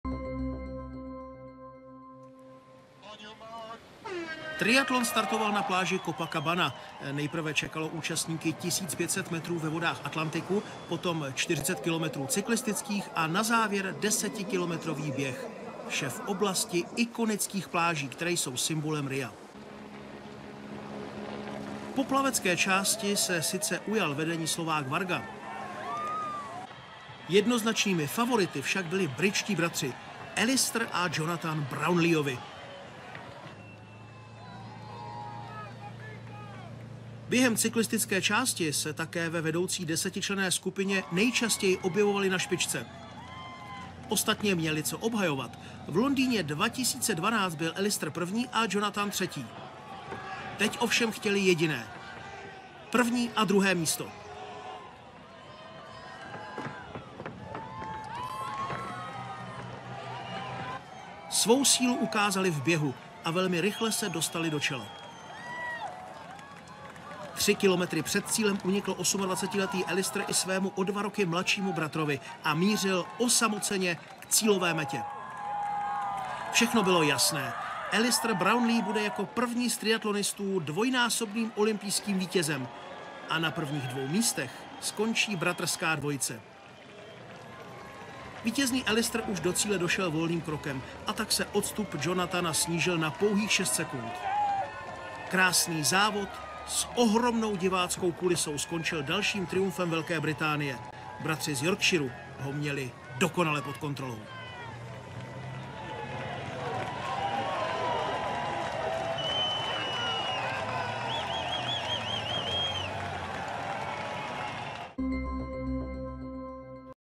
Triatlonový závod mužů na LOH Rio de Janeiro 2016
Výsledky triatlonu mužů:
1. A. Brownlee 1:45:01, 2. J. Brownlee (oba Brit.) -6, 3. Schoeman -42, 4. Murray (oba JAR) -49, 5. Pereira (Portug.) -51, 6. Van Riel (Belg.) -1:02, 7. Luis (Fr.) -1:11, 8. Mola (Šp.) -1:25.